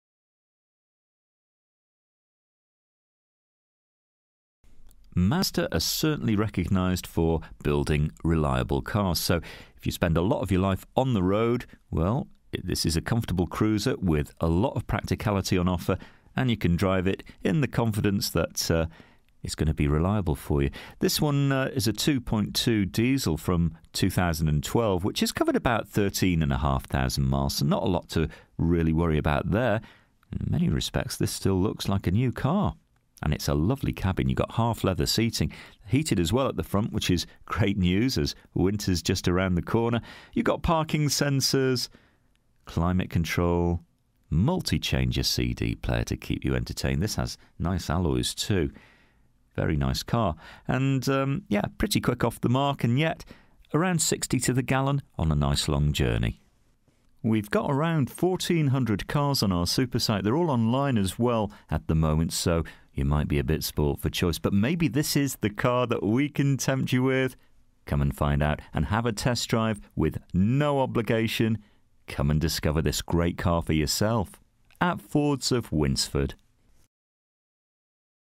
MAZDA MAZDA6 DIESEL ESTATE (2012) 2.2D [163] TAKUYA 5DR - SV12VOJ
DETAILS:
First registered: 26/03/2012
Current mileage: 13410
Registration: SV12VOJ
Colour: SILVER

FEATURES:
all round electric windows, remote central locking, half leather interior, all round airbags, climate control, pas, abs, alloy wheels, metallic paint, cd-6 multichanger, heated seats, traction control, parking control, hand book present, cruise contro

This used car is for sale at Fords of Winsford Car Supermarket in Cheshire, UK